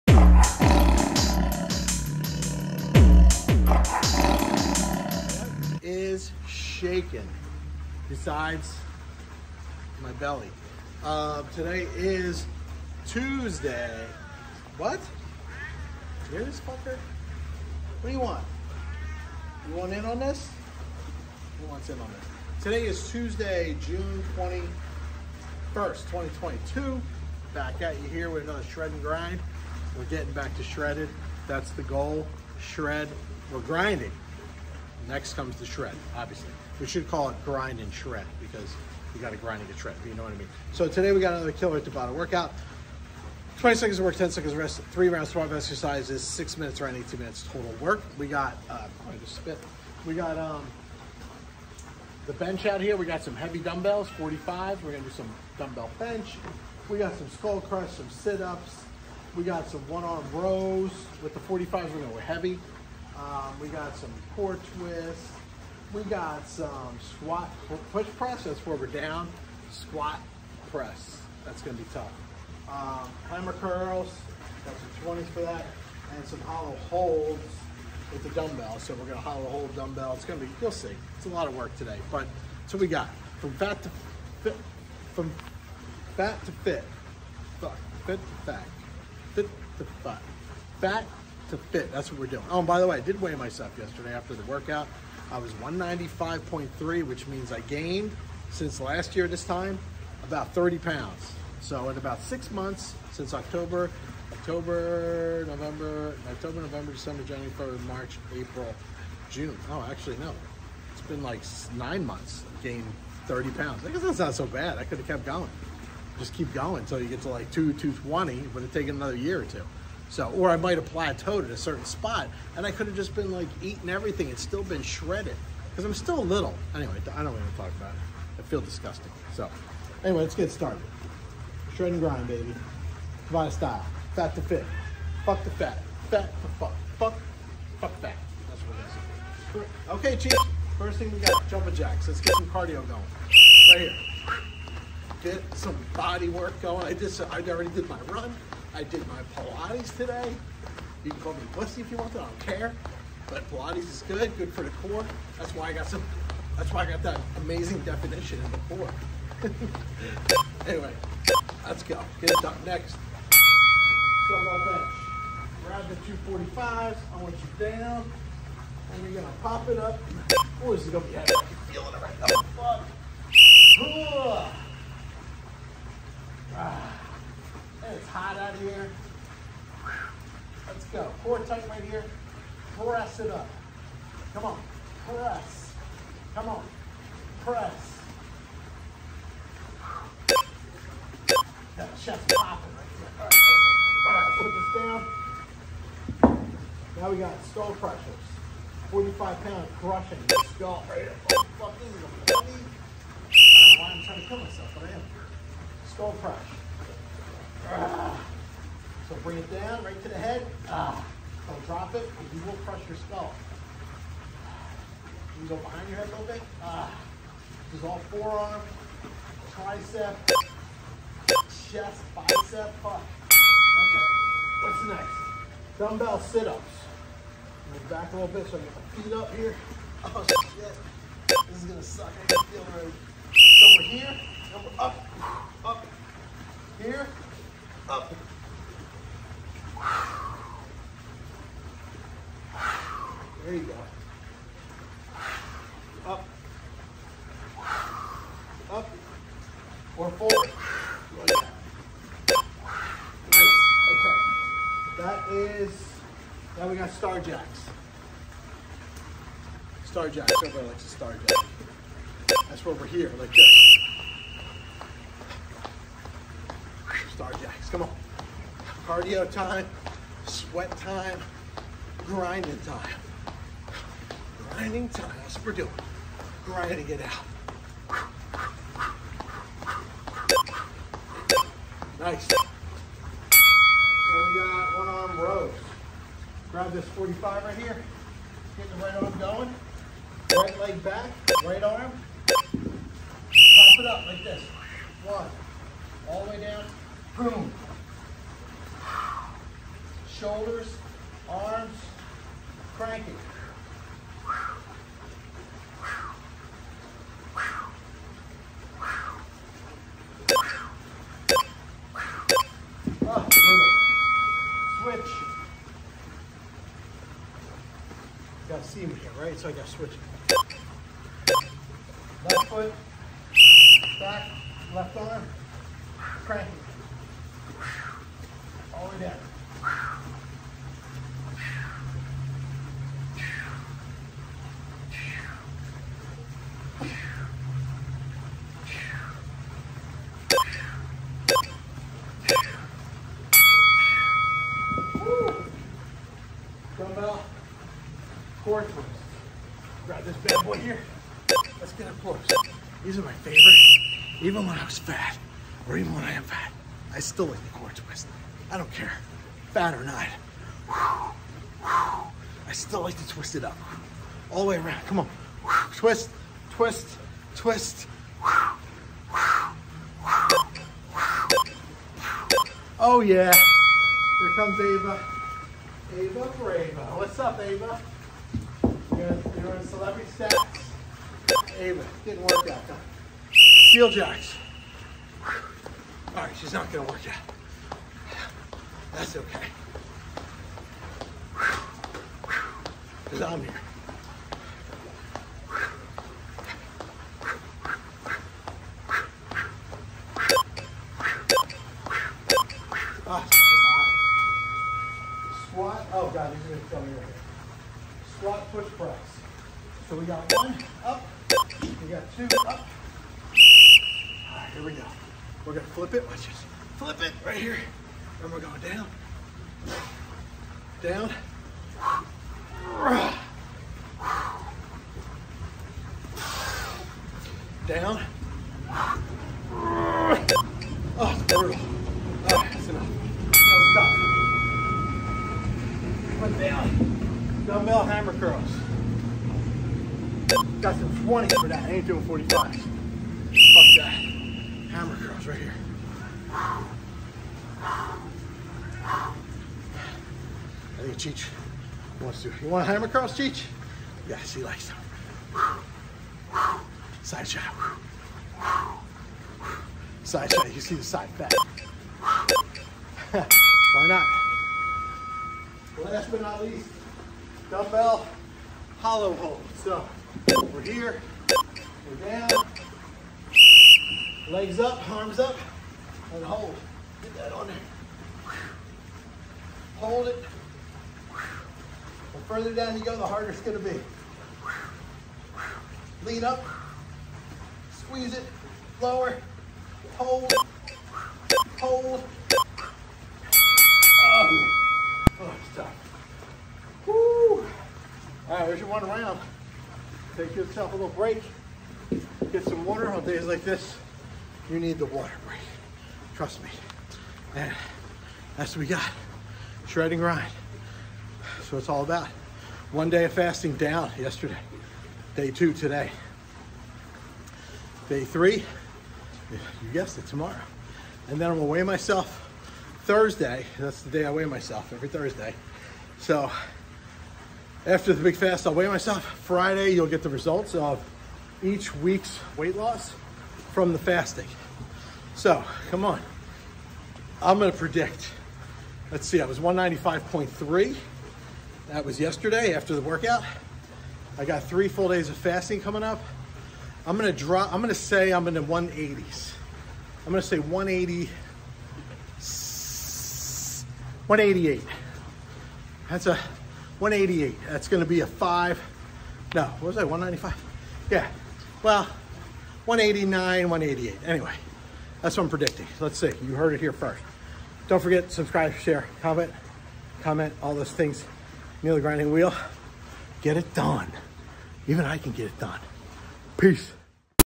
Shred and Grind Tabata Style - Tuesday Dumbbell Crushing Tabata Workout - 06/21/2022
At Home Workouts with your host and Tabata guide ... Neil the grinding wheel!!

Word for the year 2022 is:   DETERMINATION!!

195.3lbs on 6/21/22

20 sec work 10 sec rest
12 Exercises = 1 round
3 rounds

Tabata Exercises

Jumping Jacks
Dumbbell Bench Press
Dumbbell Skull Crushers
Dumbbell Situps

Star Jacks
1 Arm Dumbbell Rows - left
1 Arm Dumbbell Rows - right
Dumbbell Core Twist

Seal Jacks
Dumbbell Squat Thrusters
Dumbbell Hammer Curls
Dumbbell Hollow Holds